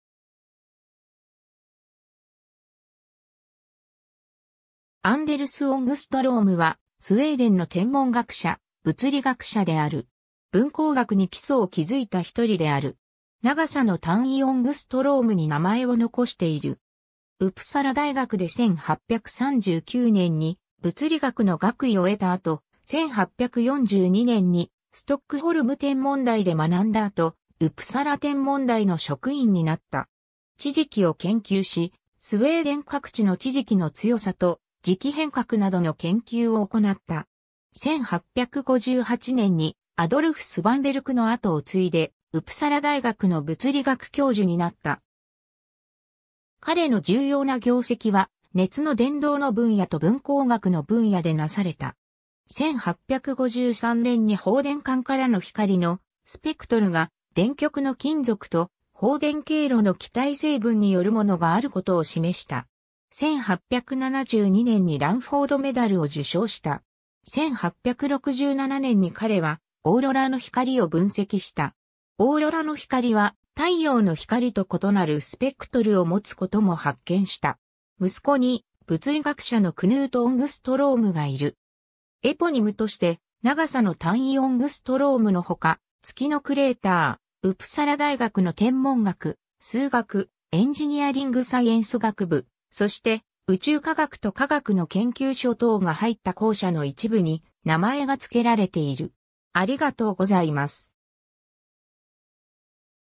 アンデルス・オングストローム
アンデルス・オングストローム（Anders Jonas Ångström [ˈan.ˈdəʂ ˈjuː.ˈnas ˈɔŋ.ˈstrøm]、1814年8月13日-1874年6月21日）は、スウェーデンの天文学者、物理学者である。
分光学に基礎を築いた一人である。
長さの単位オングストローム（1オングストローム=1×10-10 m）に名前を残している。
 ウプサラ大学で1839年に物理学の学位を得た後、1842年にストックホルム天文台で学んだ後ウプサラ天文台の職員になった。
地磁気を研究しスウェーデン各地の地磁気の強さと磁気偏角などの研究を行った。
 1858年にアドルフ・スヴァンベルクの後をついでウプサラ大学の物理学教授になった。
彼の重要な業績は熱の伝導の分野と分光学の分野でなされた。
1853年に放電管からの光のスペクトルが電極の金属と、放電経路の気体成分によるものがあることを示した。
1872年にランフォード・メダルを受賞した。
1867年に彼はオーロラの光を分析した。
オーロラの光は太陽の光と異なるスペクトルをもつことも発見した。
 息子に、物理学者のクヌート・オングストロームがいる。
 エポニムとして、長さの単位オングストローム（1Å＝1×10-10m）の他、月のクレーター（en:Ångström）、ウプサラ大学の天文学・数学・エンジニアリングサイエンス学部、そして宇宙科学と化学の研究所等が入った校舎の一部に名前が付けられている。